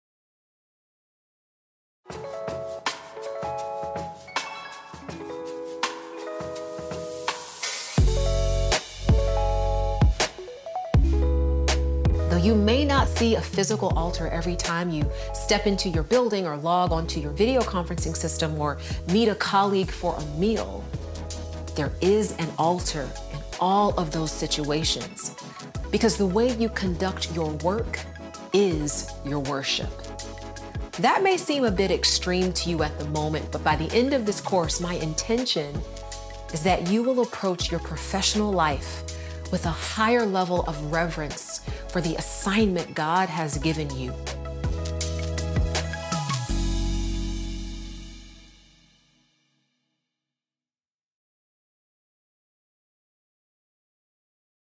Work as Worship
Description
Many of us find our work lives to be hollow and lacking in purpose, but the joy of worshipping God can be experienced even in our secularized work environment. In this six-session series, business executive and international speaker Nona Jones shares how our work carries deep spiritual significance and is directly tied to our identity as God's people. Our work is not just a job—it is a mission.